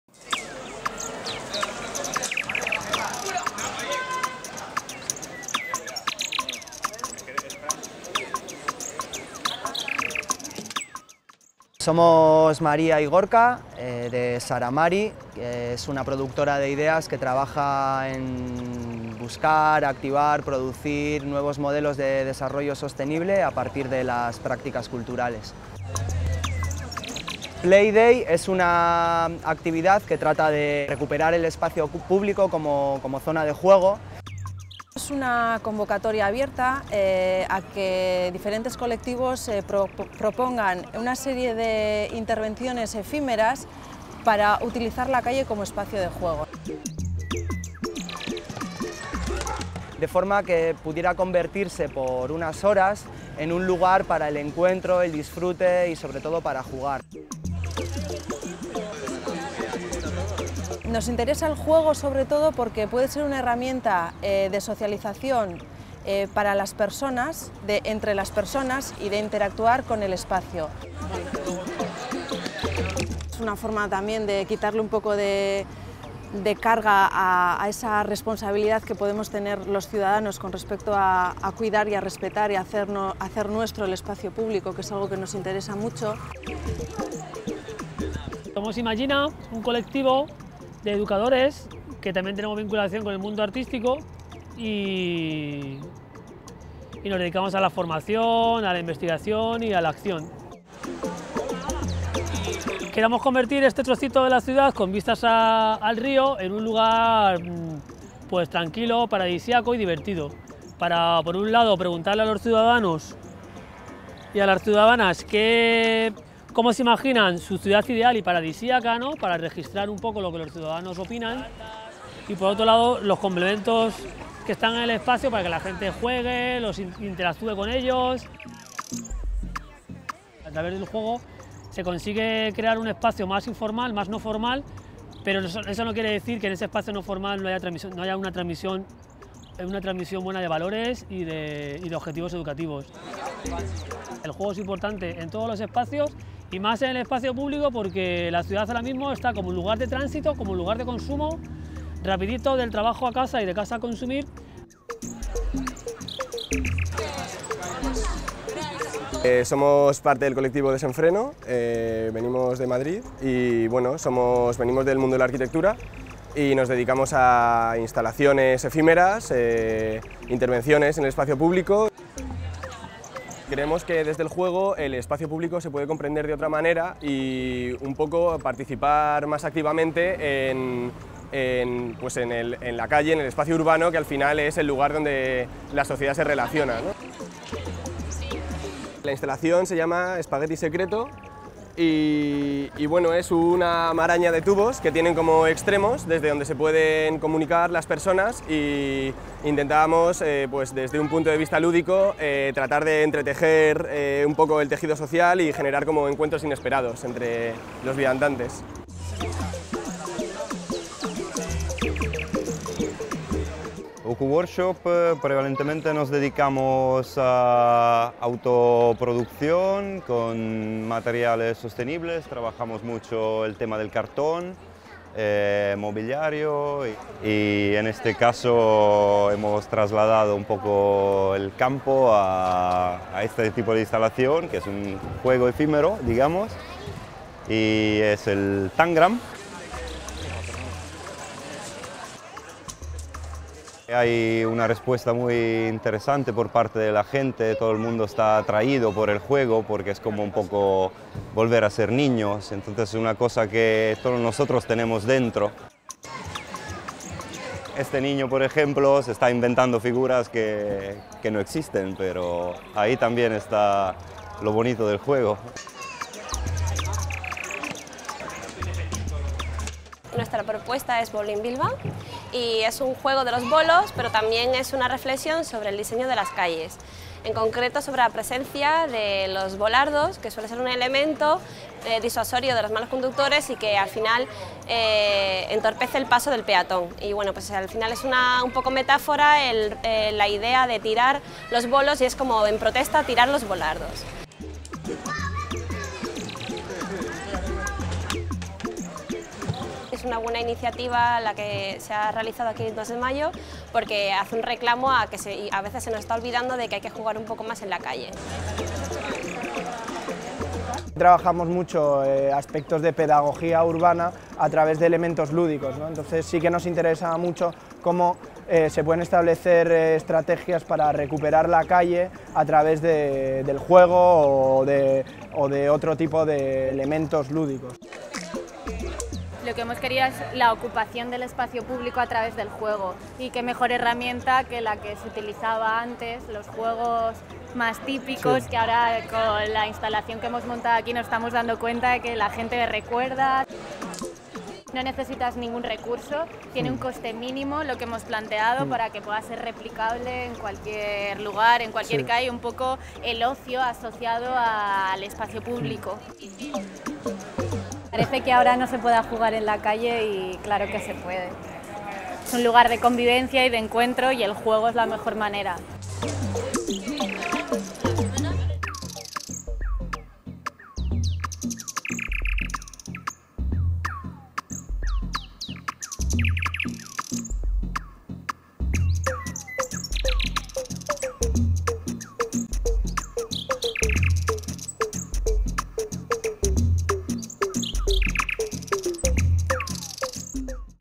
PlayDay (2012)
PLAYday es una propuesta por recuperar el juego como herramienta para la socialización, para la transformación social y física de la calle. Un día para acondicionar temporalmente el espacio público y disfrutarlo de otra manera. Una iniciativa que se presenta como una respuesta frente al modelo actual de la calle para el tránsito de vehículos y personas. Una ocasión para convertir la calle por un día en un lugar para experimentar con el espacio de forma libre y creativa, dotando de espacio a la imaginación.